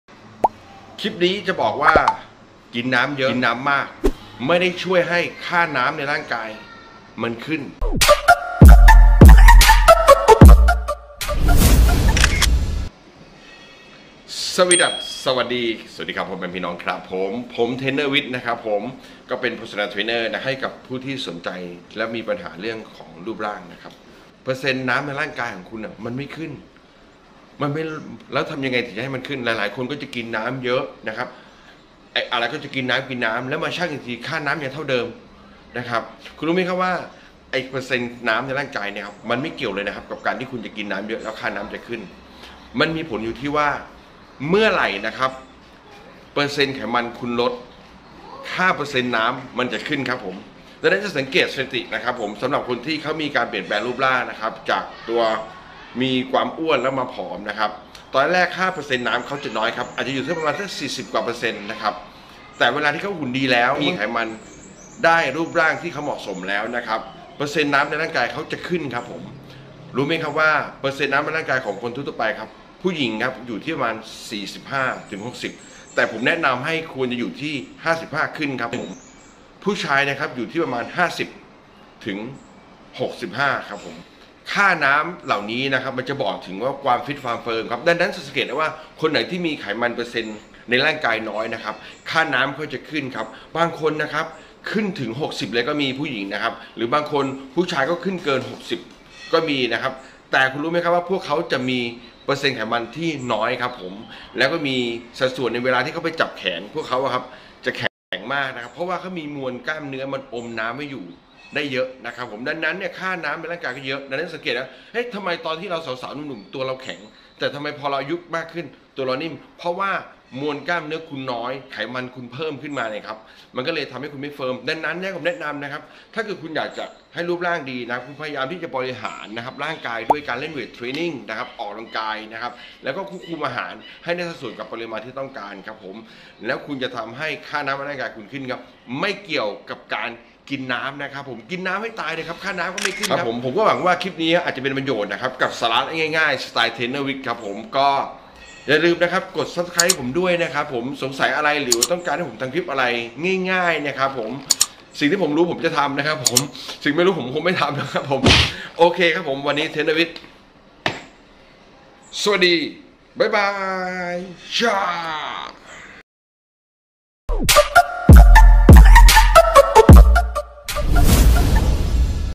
ดื่มน้ำมากค่า%น้ำในร่างกายขึ้นจริงหรือ?
พระนามรัชกาลคือความงานแน่นของมวลกล้ามเนื้อหากเมื่อไหร่สัดส่วนไขมันมีมากมายร่างกายก็จะมีน้อย
 การที่จะทำให้ค่าน้ำขึ้นได้คือต้องลดไขมันในร่างกาย
TrainerVic